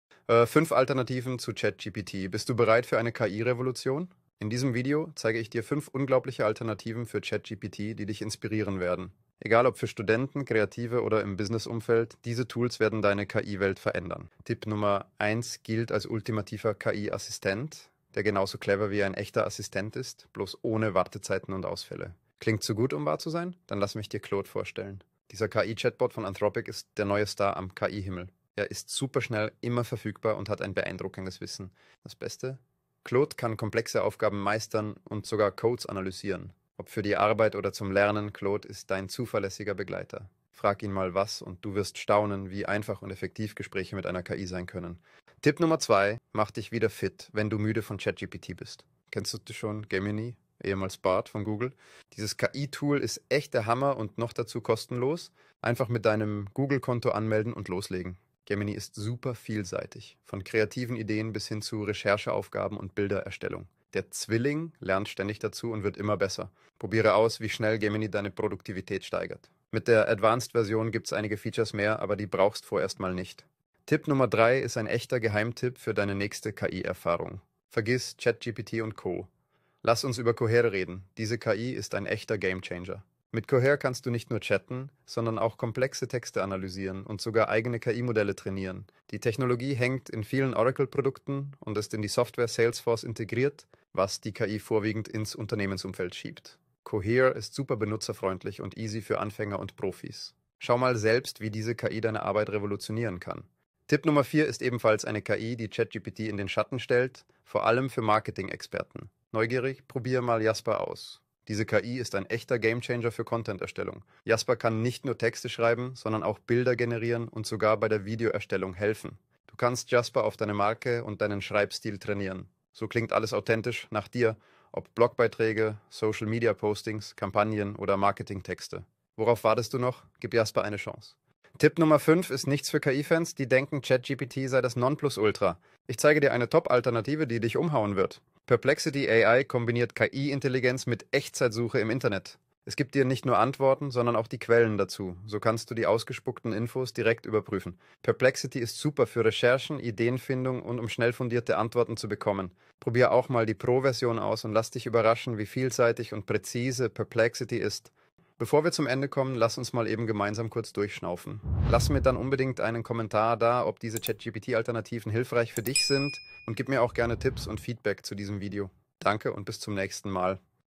5 kostenlose ChatGPT-Alternativen | Kennst du alle?
Du nutzt regelmäßig Chat GPT, um an Informationen zu kommen? Aber irgendwie bist du nicht mehr ganz so zufrieden mit den Ergebnissen oder hast dich an der Oberfläche satt gesehen? Dann wird es Zeit für kostenlose ChatGPT-Alternativen. Schau mal, ob du eines dieser fünf KI Tools besser findest oder es Infos für dich besser findet.

00:00 Intro
00:14 Claude AI
00:45 Google Gemini
01:13 Cohere
01:43 Jasper AI
02:11 Perplexity AI